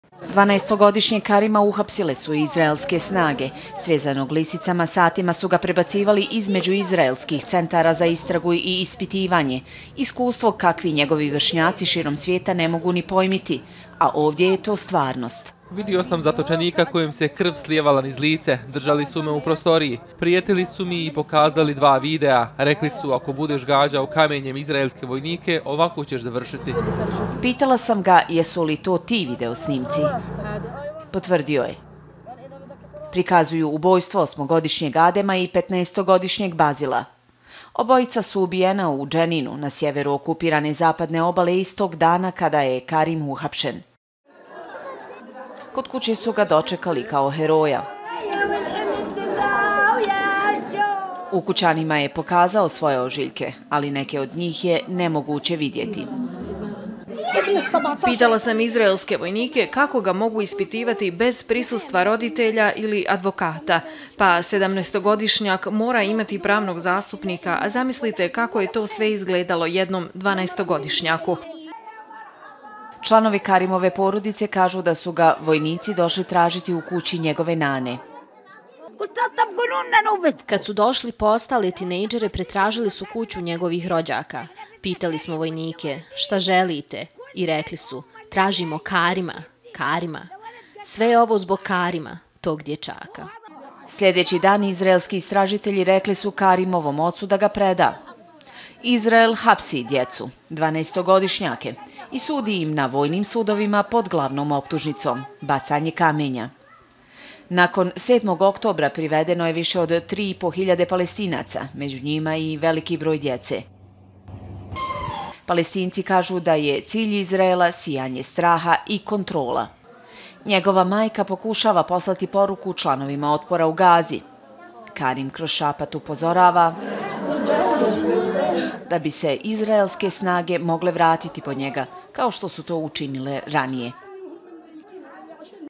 Hapšenje palestinske djece: Izraelci prijetili 12-godišnjaku u pritvoru
Izraelske vlasti satima su držale palestinskog dječaka u istražnom centru na okupiranoj Zapadnoj obali.
Dvanaestogodišnji Karim Al Jazeeri je prepričao iskustvo s kojim se suočio tokom ispitivanja - od zastrašivanja do prijetnji.